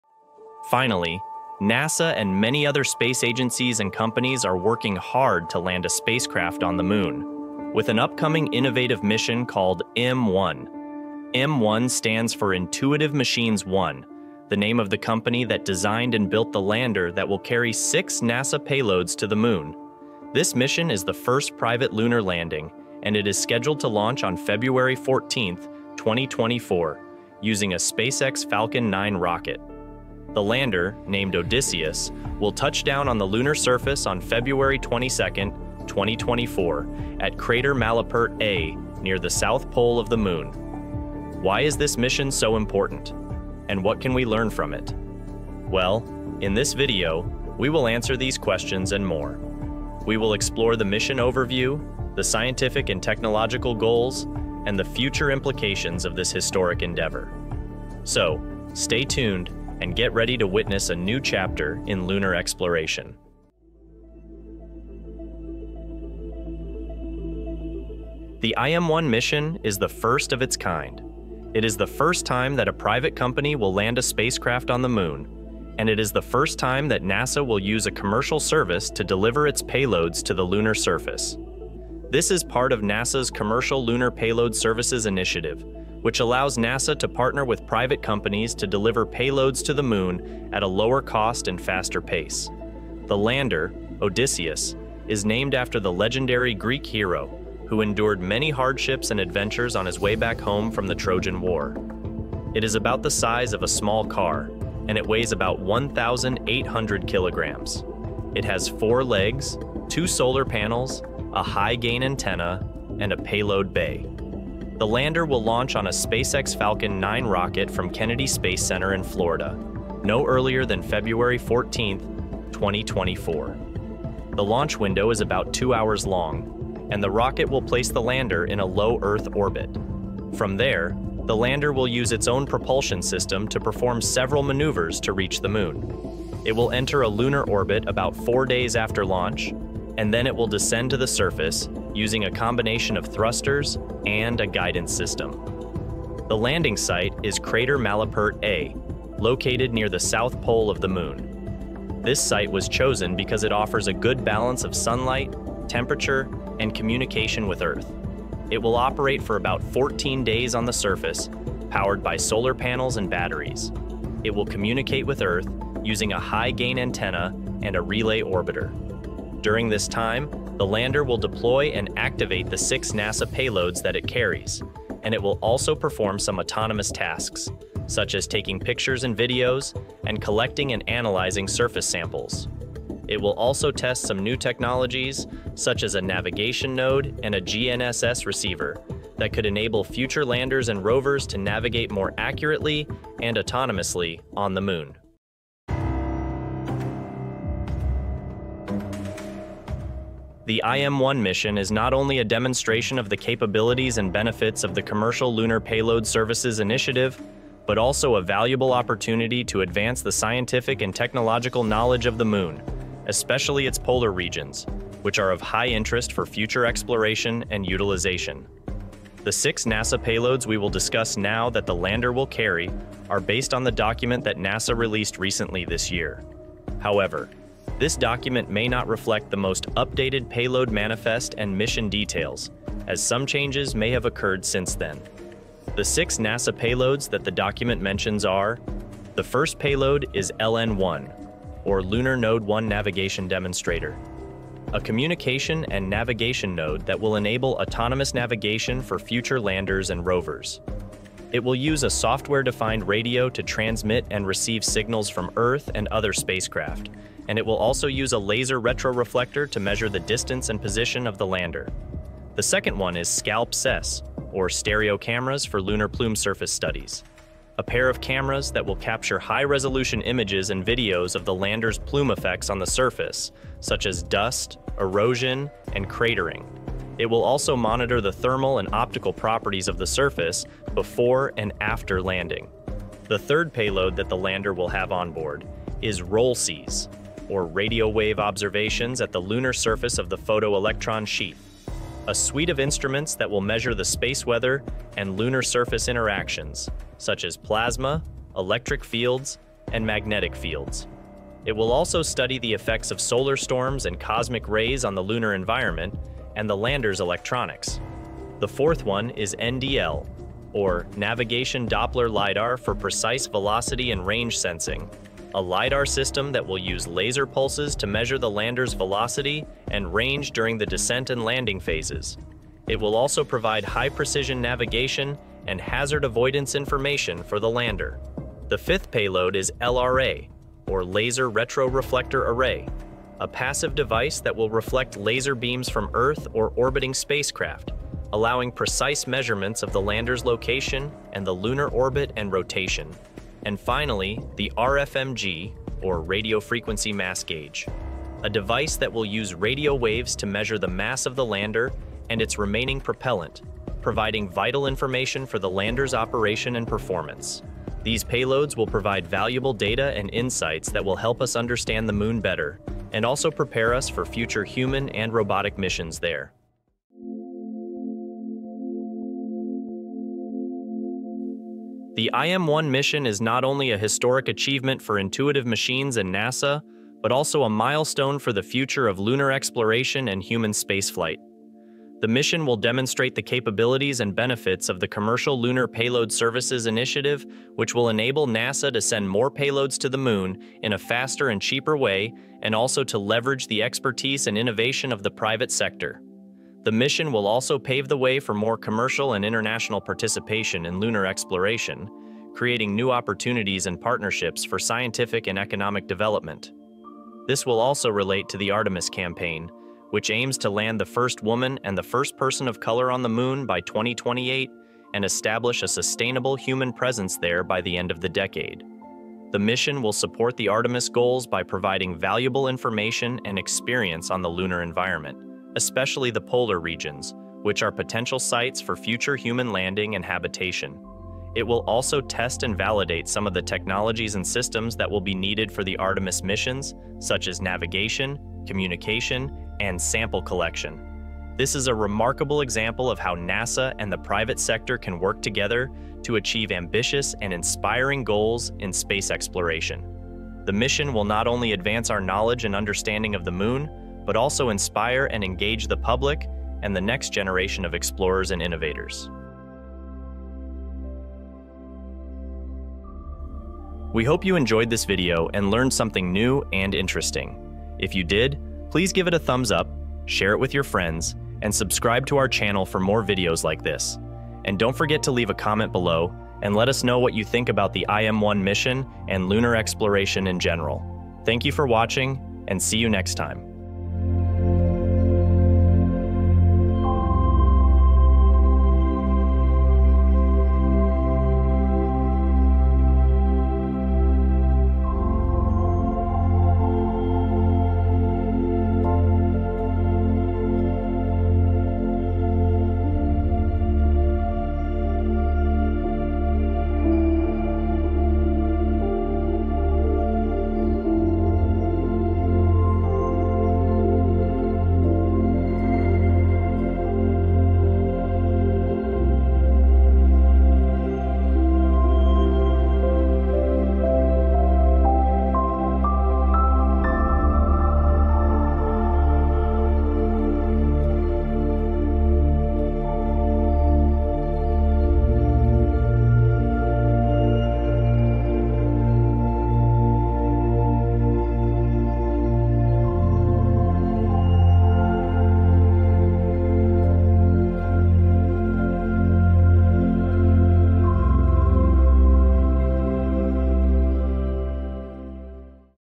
Watch Out! SpaceX & Intuitive Machines are Launching a Lander to the Moon
On Feb. 14, 2024, a private company called Intuitive Machines will launch a lander to the Moon, carrying six NASA payloads that will perform various scientific and technological experiments on the lunar surface. This is the first private lunar landing, and it is part of NASA’s Commercial Lunar Payload Services initiative, which allows NASA to partner with private companies to deliver payloads to the Moon at a lower cost and faster pace. In this video, we will explore the mission overview, the scientific and technological goals, and the future implications of this historic endeavor.

Chapters:
00:00 Introduction
01:03 Mission Overview
03:15 Scientific and Technological Goals
06:06 Future Implications
07:43 Outro
08:03 Enjoy